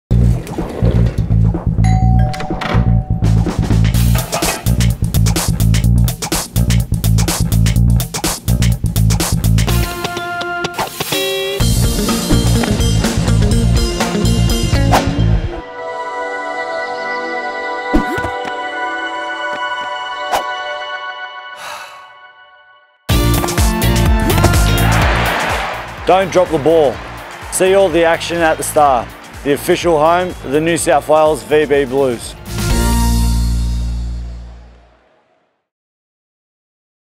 State Of Origin 2017
Don't drop the ball. See you at The Star this Origin, home of the NSW VB Blues. Players - Aaron Woods, Andrew Fafita, Jack Bird, Jarryd Hayne, Body Cordner, Josh Jackson, James Tedesco and Jack De Belin. Featured locations - The Darling's Adored Suite, Balla, The Darling Spa, The Star "We Bleed Blue" Letters and 24/7 Sports Bar.